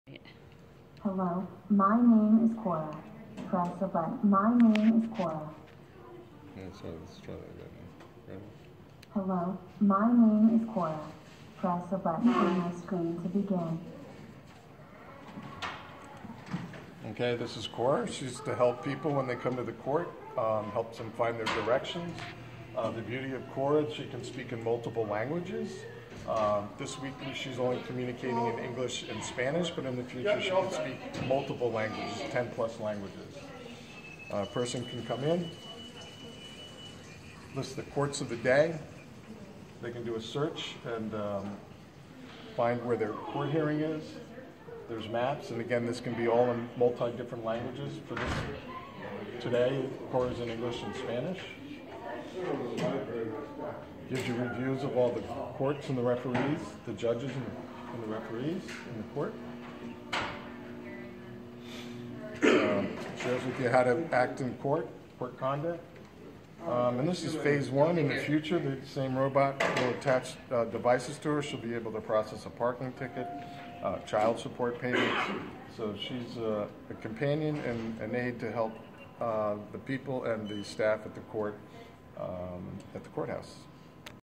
Robot greeter at Ottawa County Courthouse
An artificial intelligence robot is being tested this week at the Ottawa County Courthouse in Grand Haven